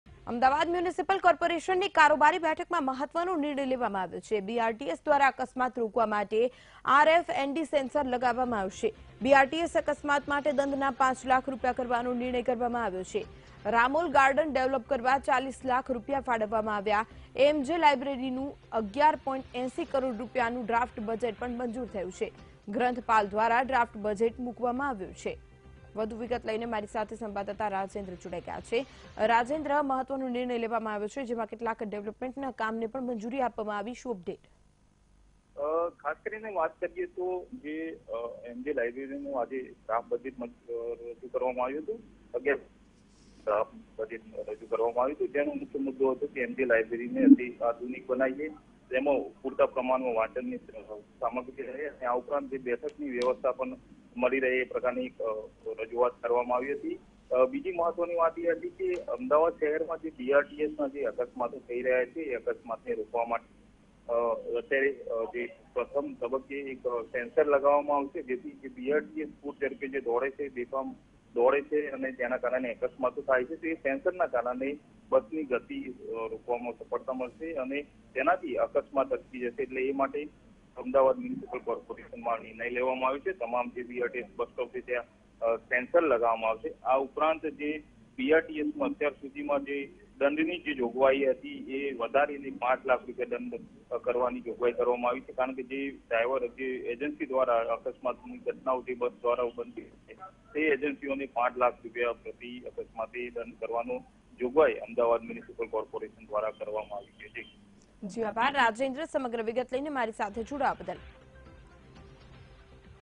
યમદૂત બનીને શહેરમાં દોડતી BRTS બસને લગામ કસવા AMC લાવી રહ્યું છે આ નવી સિસ્ટમ | Vtv News
યમદૂત બનીને શહેરમાં દોડતી BRTS બસને લગામ કસવા AMC લાવી રહ્યું છે આ નવી સિસ્ટમ.